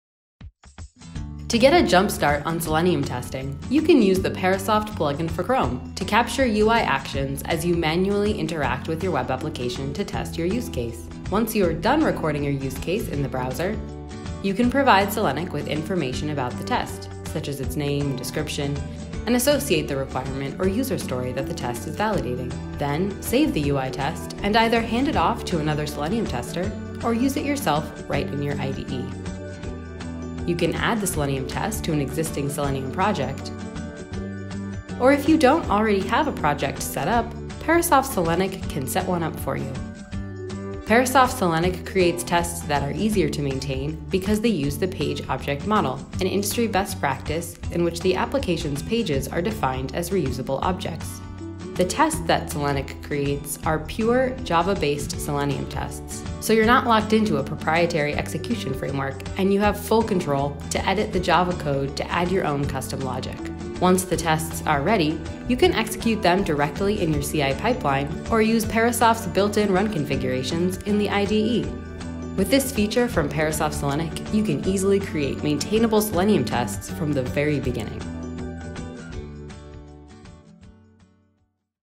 Create Smarter Selenium Tests with the Page Object Model | Parasoft Selenic
Learn how to record your UI interactions with Parasoft Selenic and get a jump start on creating maintainable Java based Selenium tests using the Page Object Model.

To get a free trial of Parasoft Selenic and check it out yourself, go (Once you have access to your trial environment, you can watch this video for a step-by-step guide on how to capture a UI recording in the Selenic trial environment